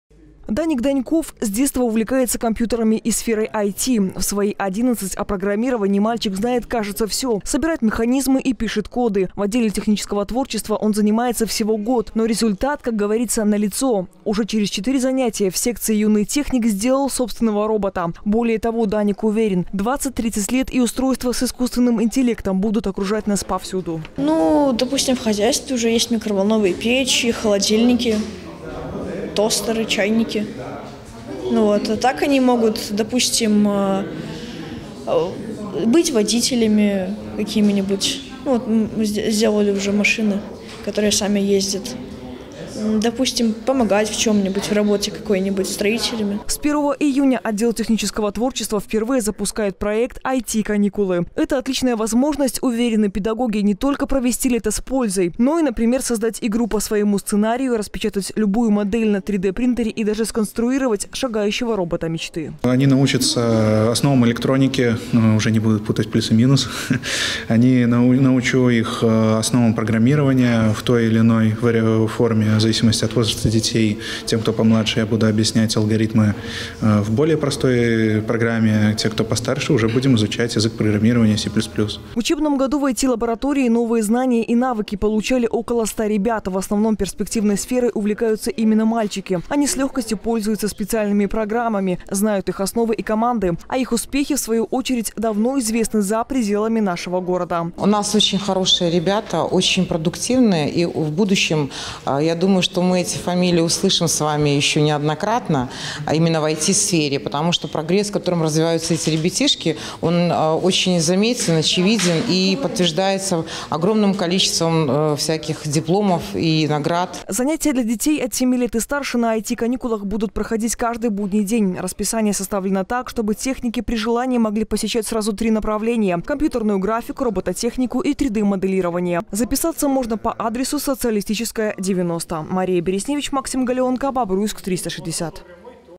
"IT-каникулы"
Специалисты отдела технического творчества задумали  провести для детей «IT-каникулы». «Робототехника», «3-Dмоделирование» и «Компьютерная графика».